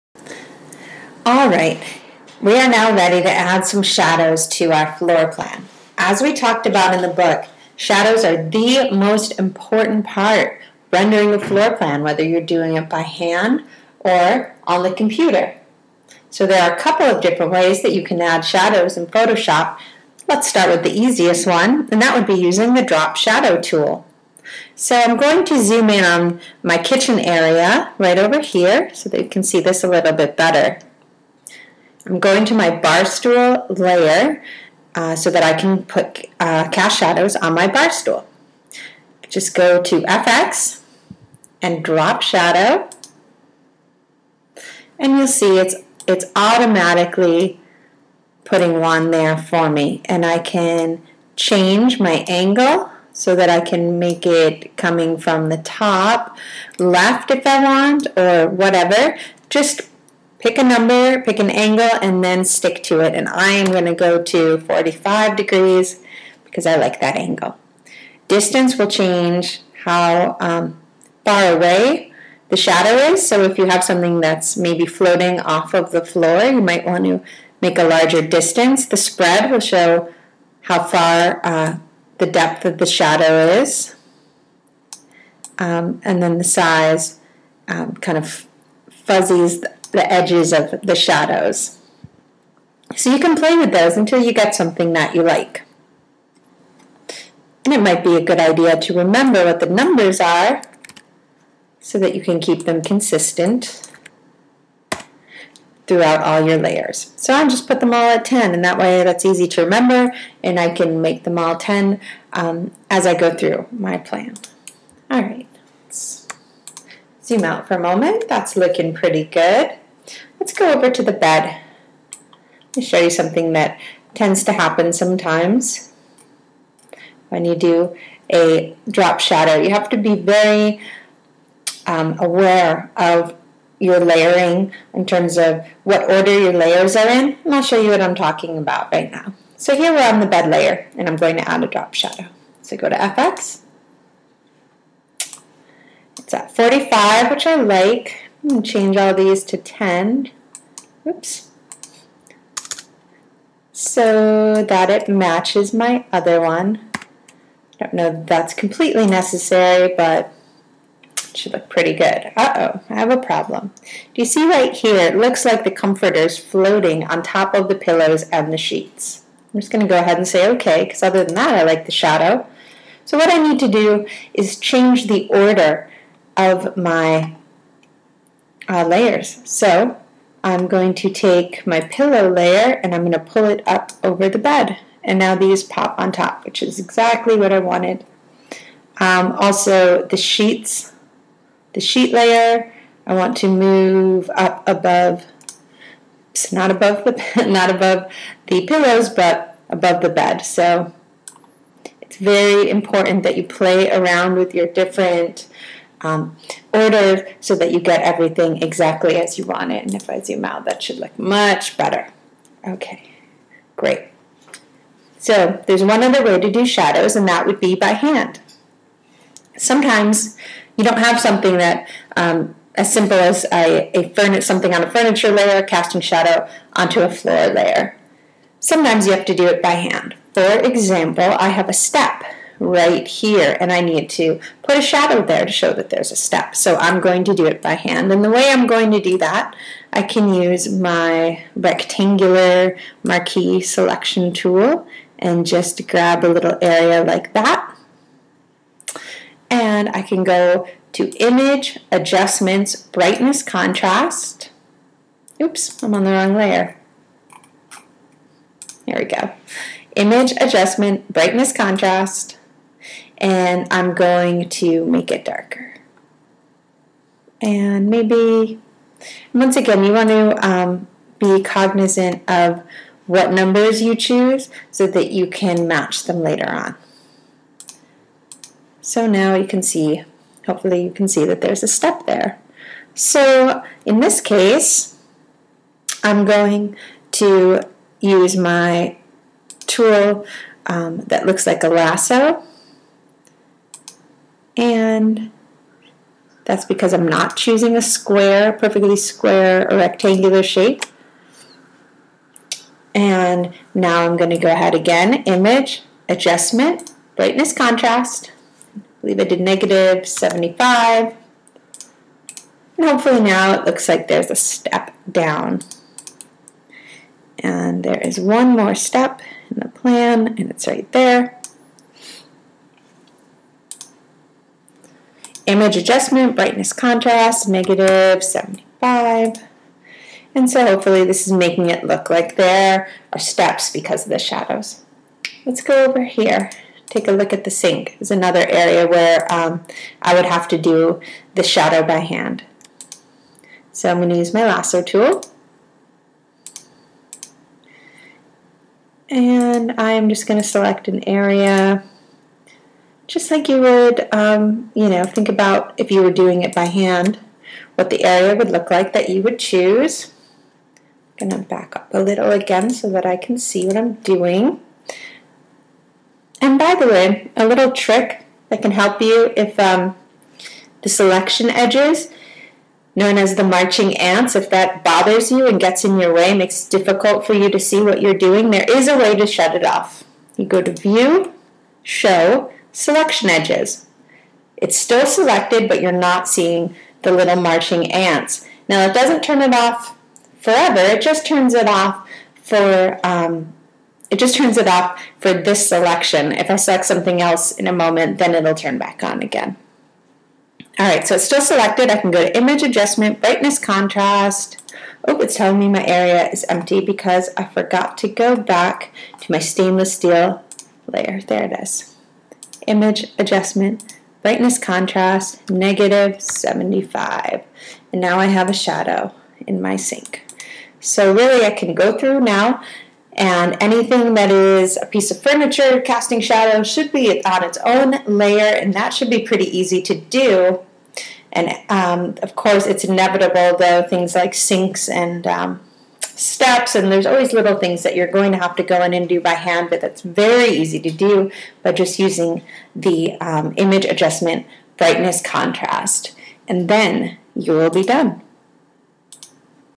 Photoshop Floor Plan How to Create Cast Shadows
How to create cast shadows on your floor plan in photoshop using the "drop shadow" tool or by using the gradient tool.